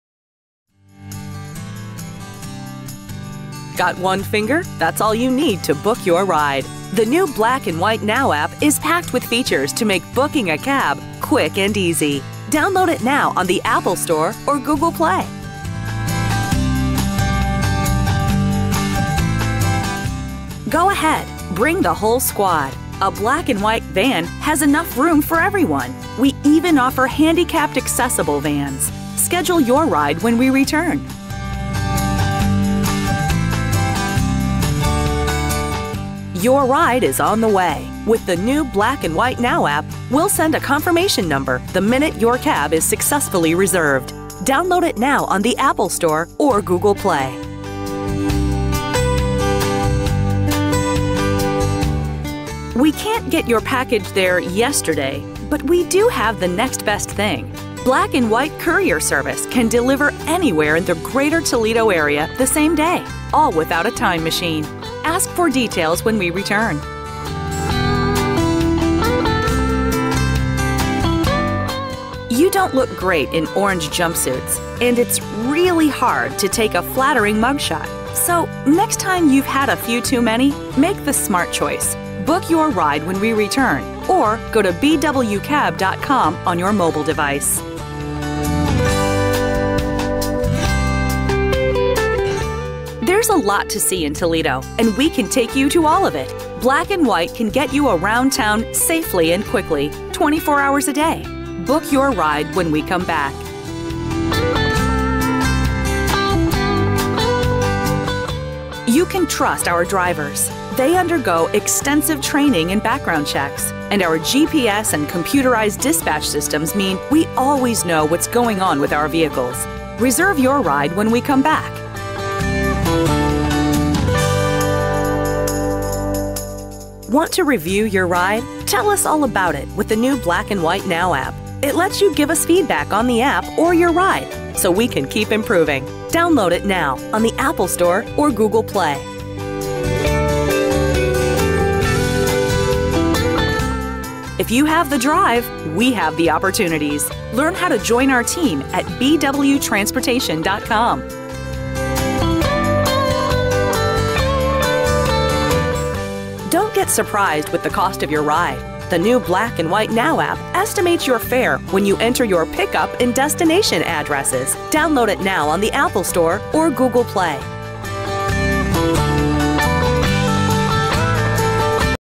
Black and White Transportation On Hold Marketing - BusinessVoice.com
BusinessVoice created this On Hold Marketing production for Black and White Transportation in Toledo, Ohio. Learn how Caller Experience Marketing can work for your company. or call 419/473-9000.